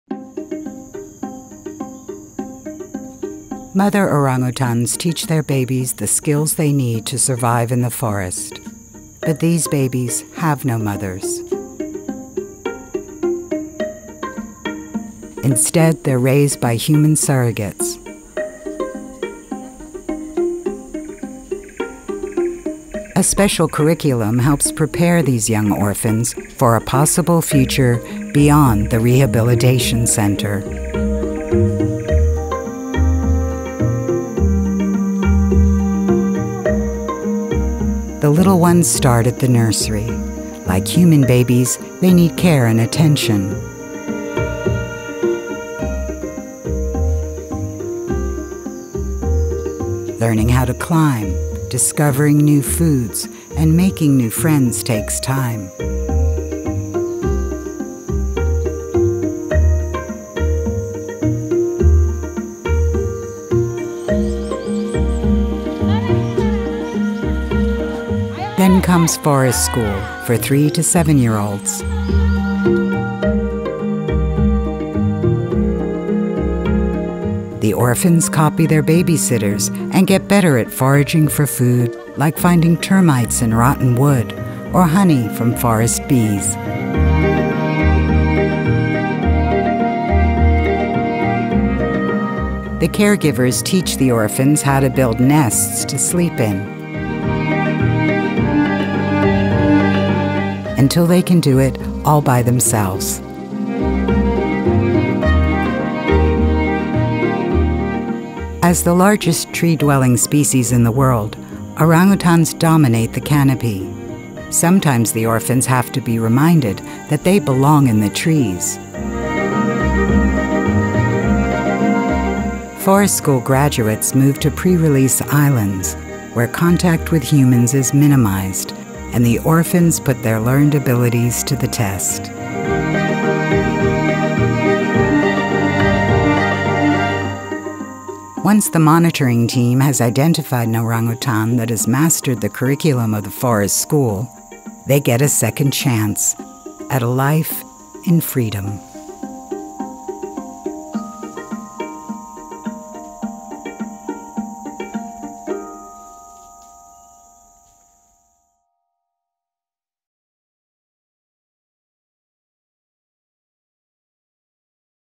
Critical Life Skills
Baby orangutans learn all the forest skills they need from their mothers. In Forest School young orphaned orangutans learn skills from babysitters and through watching other orangutans hence these skills will be vital once released to the wild. Check this out of how we preparing the orangutans to become independent in the wild!